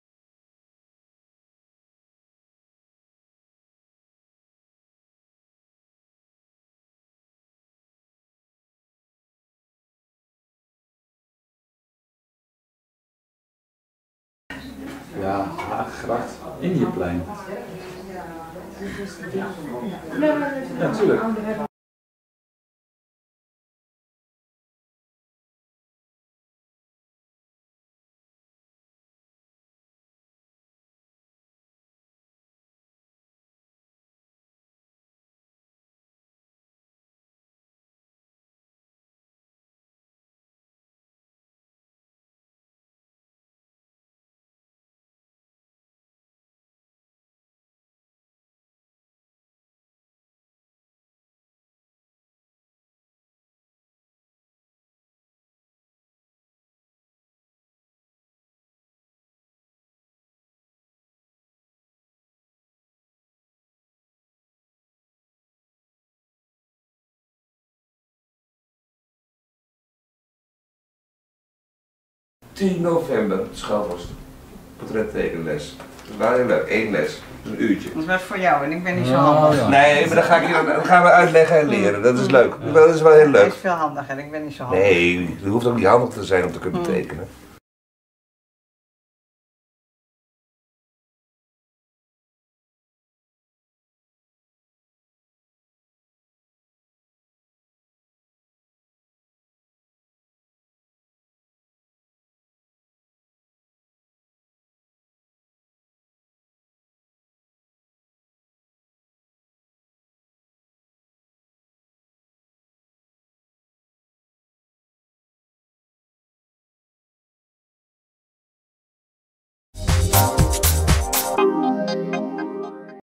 Een kijkje bij Aschwin Huiskes en Rob Koenders
Was het vorig jaar nog één grote regenpartij. Dit jaar deed de zon extra haar best om de laatste zomerstralen aan de Atelierroute te geven. Heel veel Almeloërs, maar ook van daar buiten pakten hun fietsje of de benenwagen om een flink aantal ateliers te bezoeken.